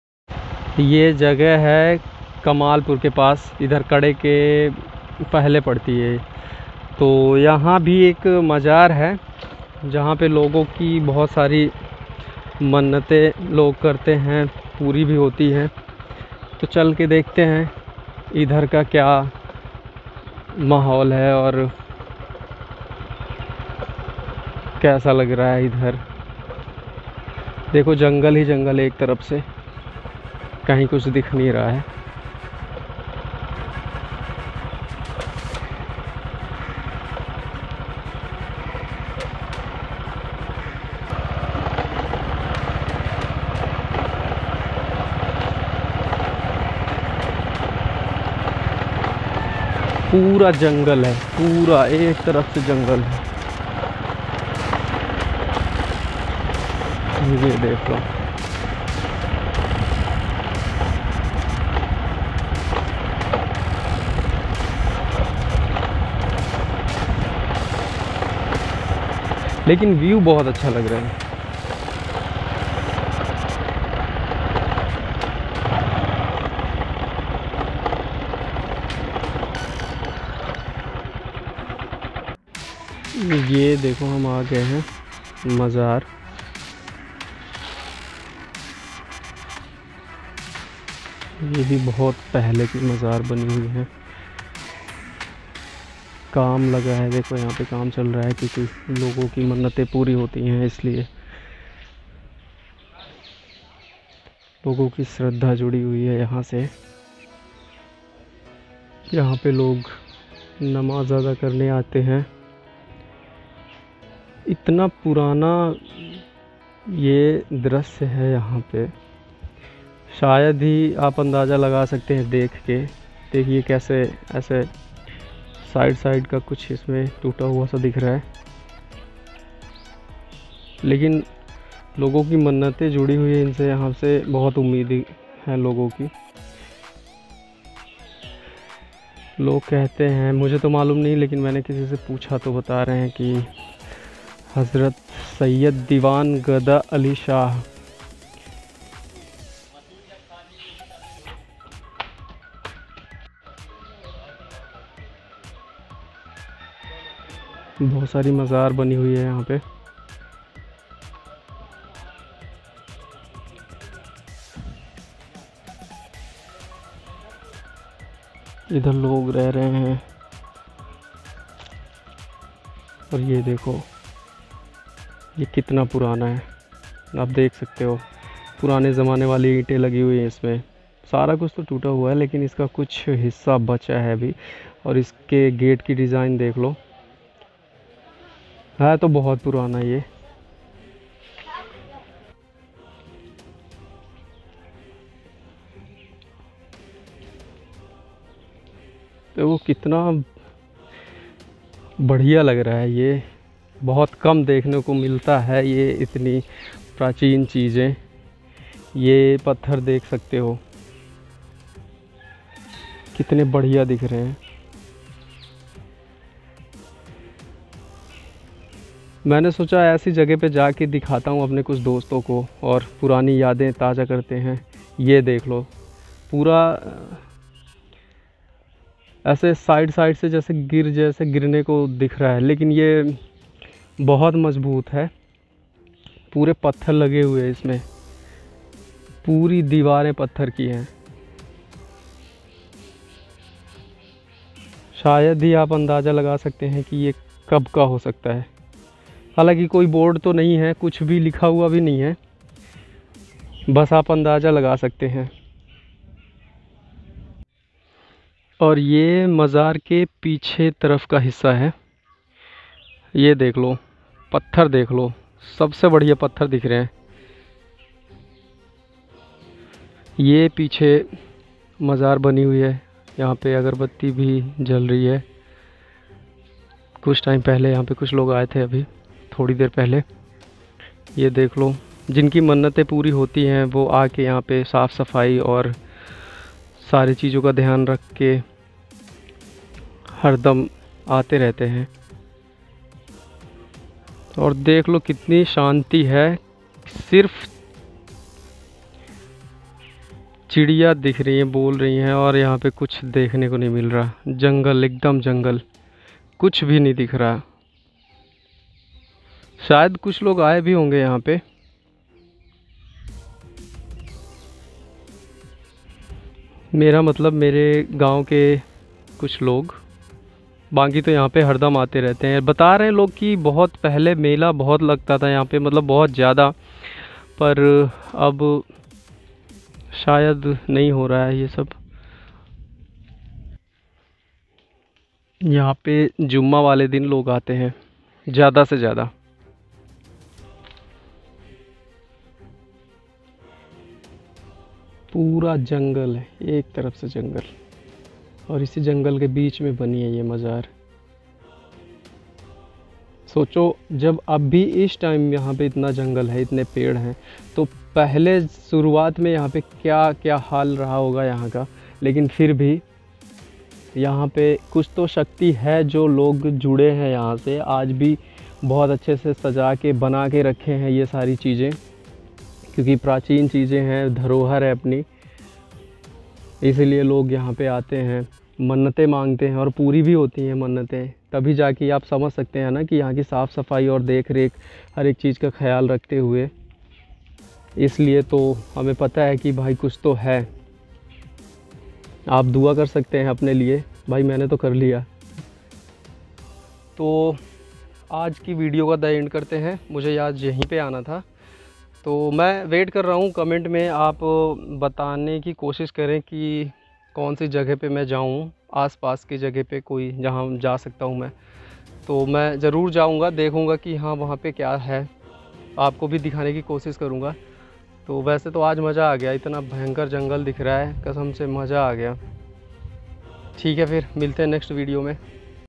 my second vlog baba ki mzar || second vlogs is very hard || in dangerous forest || sonu saigal,
my second vlog second vlogs is very difficult because forest is very dangerous . aur iski location hai . Kade ke bagal me akbar pur gavn ke age jungle hi junglehai uske bich me baba ki mzar hai ap kabhi bhi jao to smbhal ke jao .best of luck aur ek bat ki waha pe logo ki mnnate puri hoti hai.